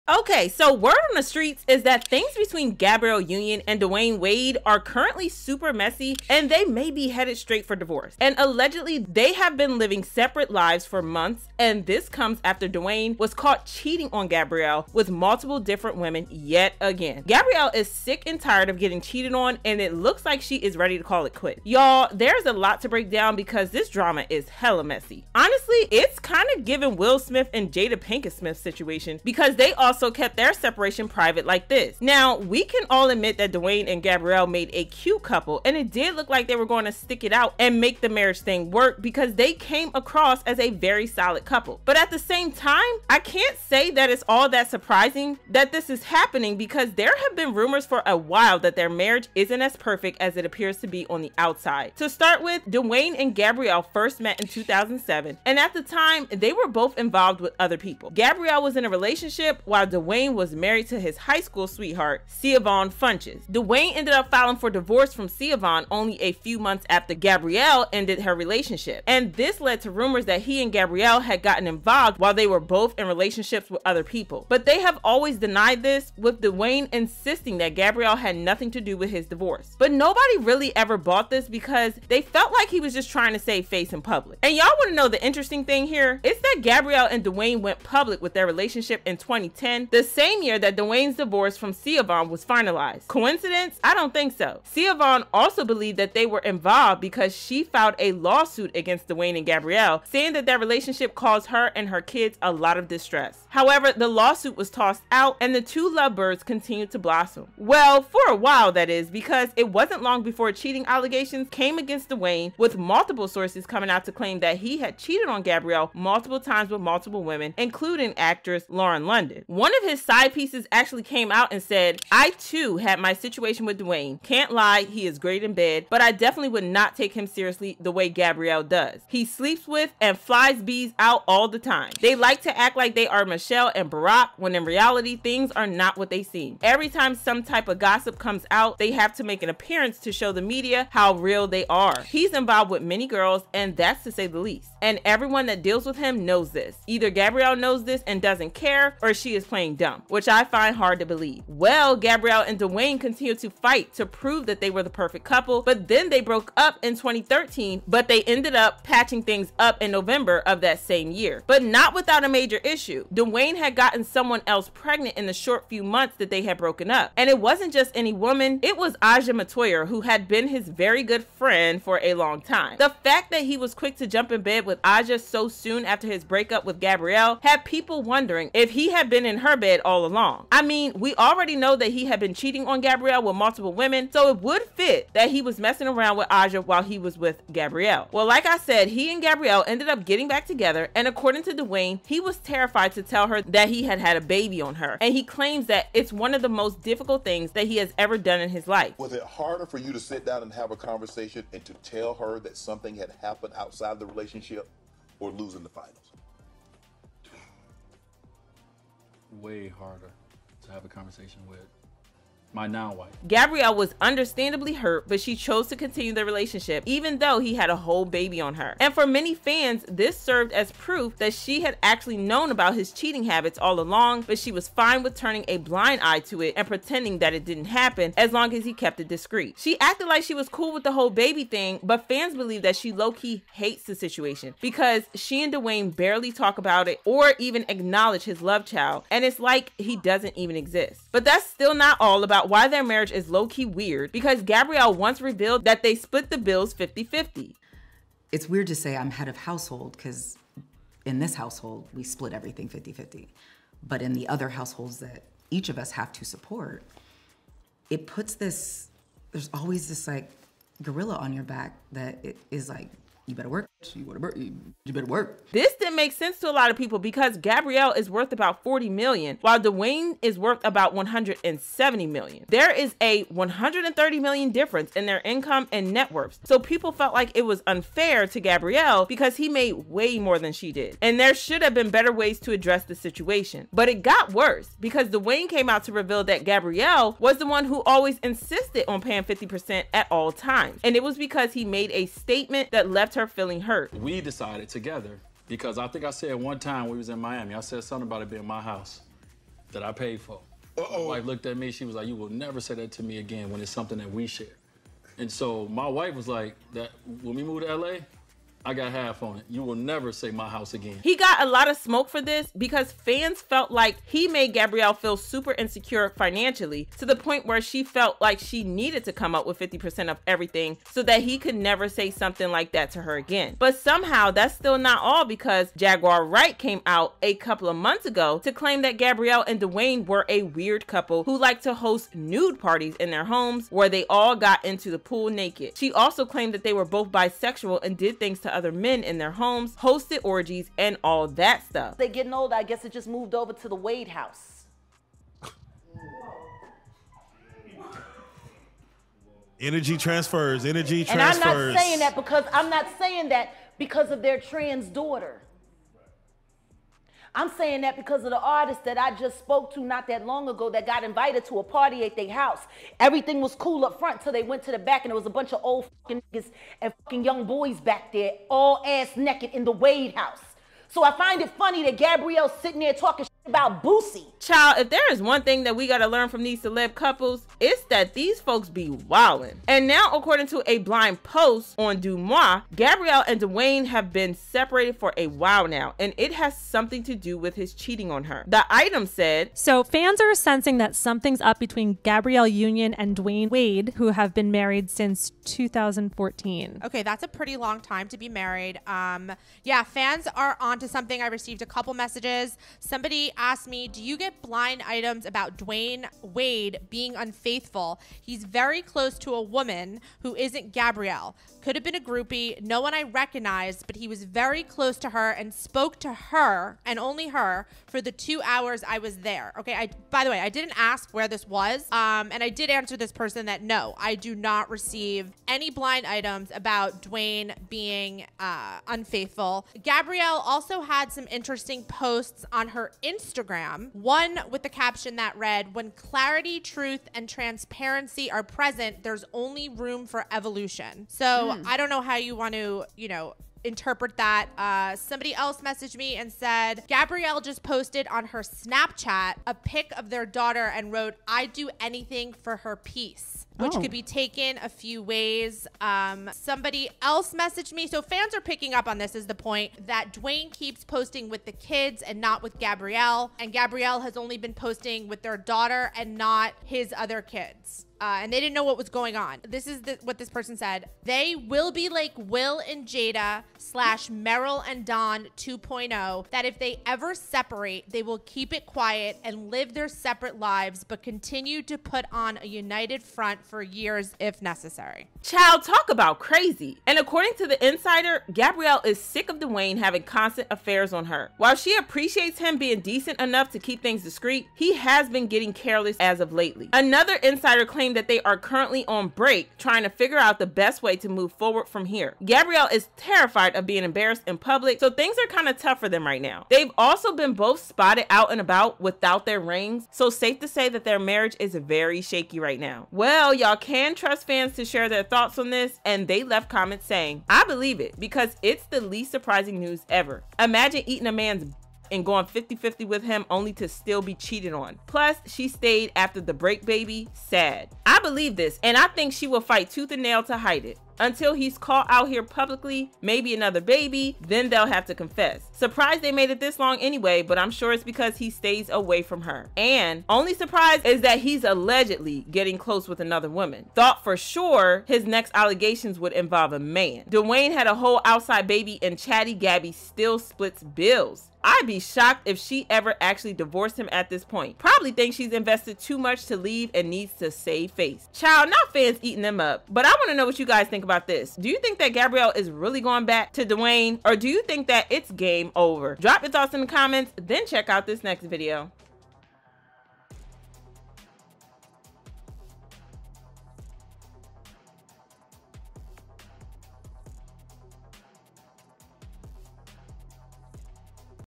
Gabrielle Union CONFRONTS Dwayne Wade For Cheating On Her
Okay, so word on the streets is that things between Gabrielle Union and Dwyane Wade are currently super messy and they may be headed straight for divorce.

 And allegedly they have been living separate lives for months, and this comes after Dwayne was caught cheating on Gabrielle with multiple different women yet again. Gabrielle is sick and tired of getting cheated on, and it looks like she is ready to call it quits.

Y’all, there’s a LOT to break down because this drama is hella messy!